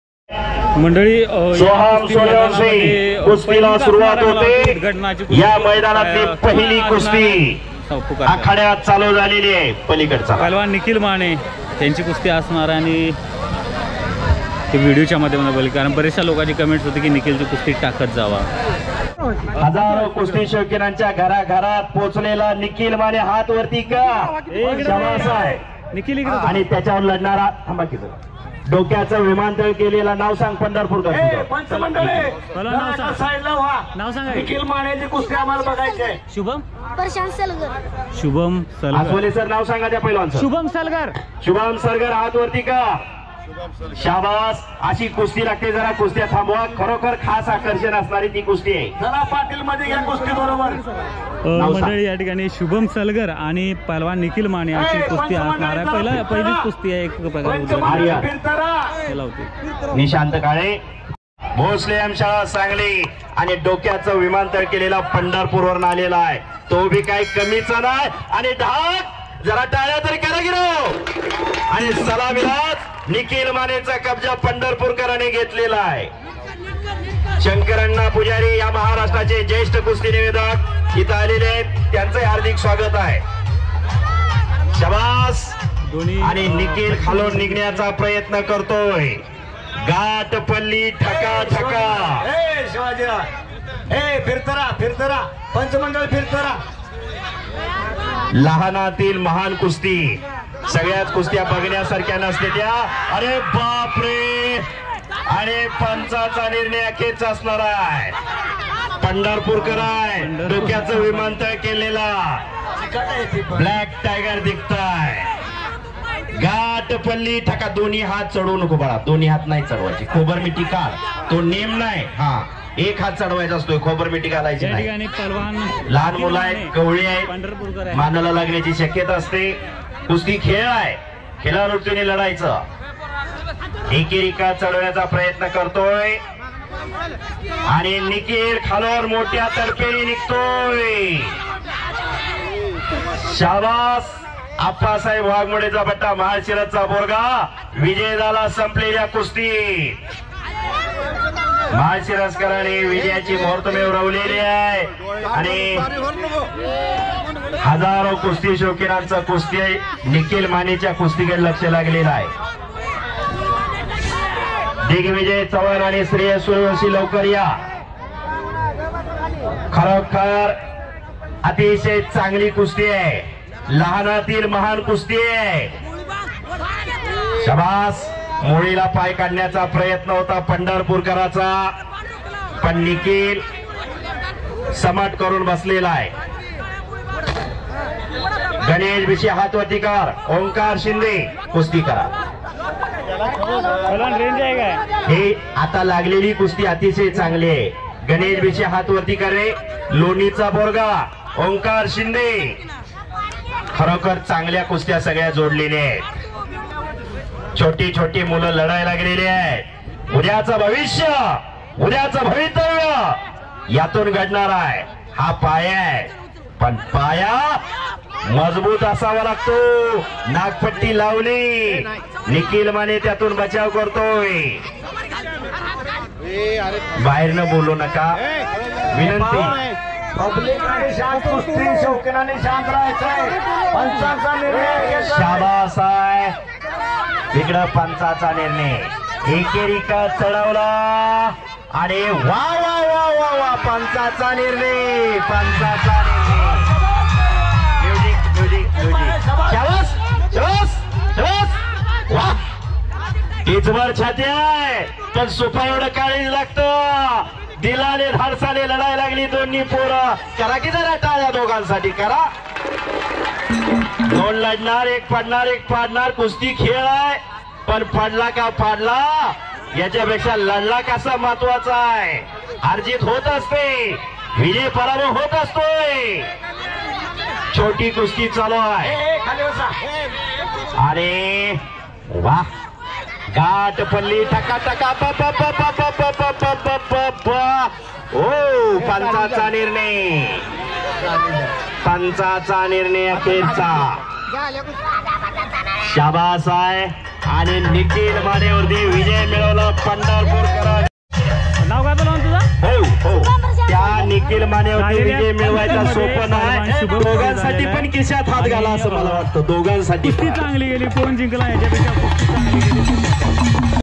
पै निखिल माने विरूद्ध पै शुभम सलगर | Nikhil Mane Vs Shubham Salgar | Nimsod Kusti Maidan
भोसले व्यायाम शाळा सांगली चा छोटा व्हायरल पैलवान निखिल माने विरूद्ध पंढरपूरचा शुभम सलगर अशी प्रेक्षणीय कुस्ती होती खुप सुंदर कुस्ती झाली झालेल्या कुस्तीमध्ये पैलवान शुभम सलगार विजयी झाले आहे